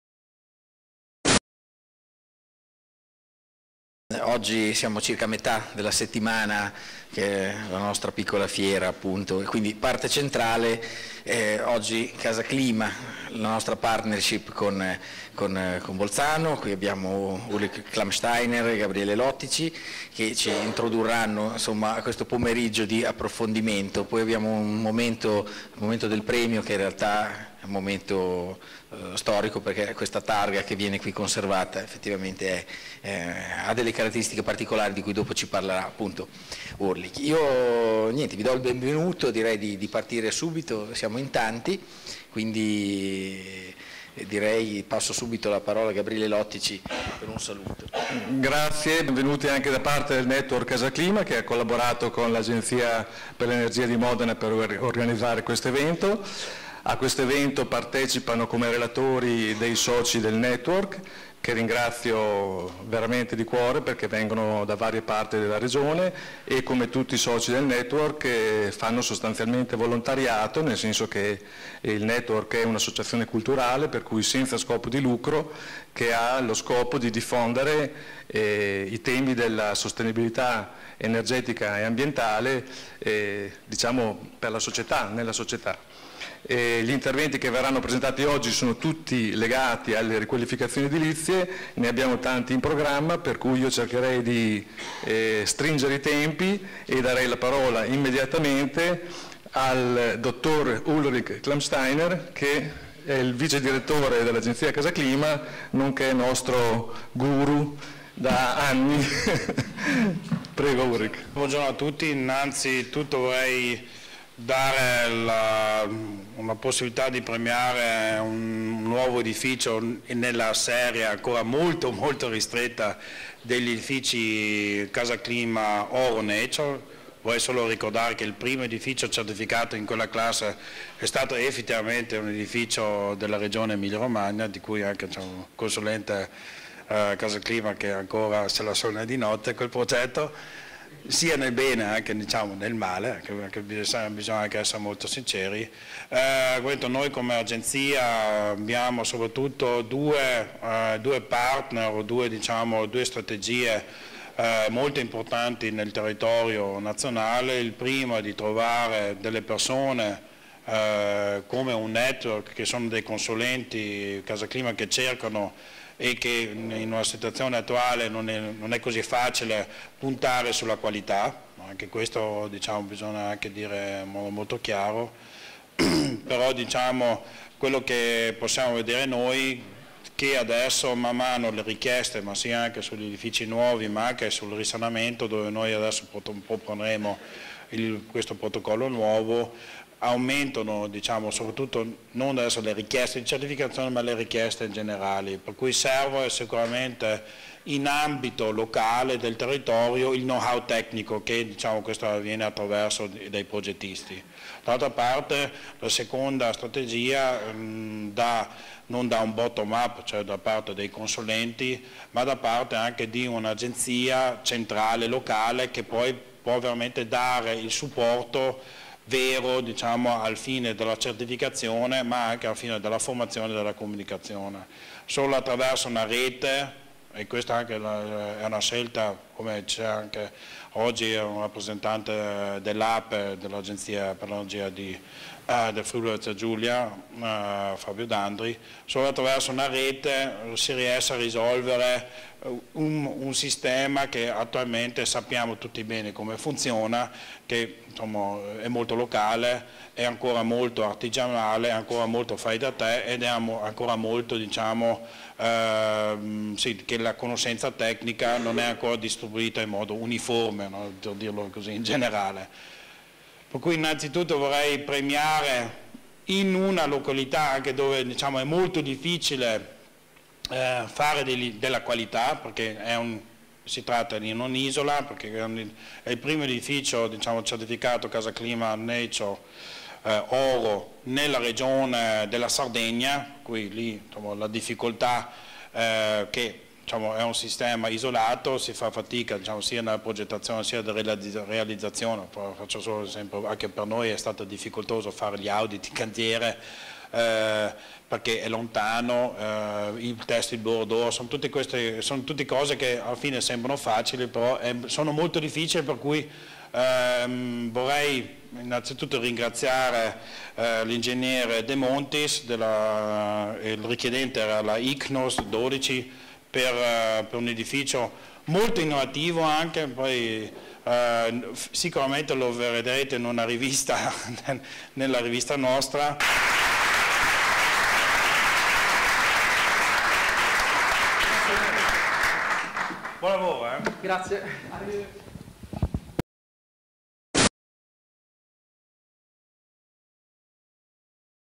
"Risanare, riqualificare, restaurare e ricostruire con efficienza energetica" - Apertura lavori
Settimana della bioarchitettura e della domotica - Apertura lavori del Seminario Tecnico "Risanare, riqualificare, restaurare e ricostruire in sicurezza con efficienza energetica e qualità". Interventi di Piergabriele Andreoli (Dir. AESS Modena), Ulrich Klammsteiner (Vicedirettore Agenzia CasaClima BZ), Gabriele Lotti (Pres. CasaClima Network Emilia-Romagna)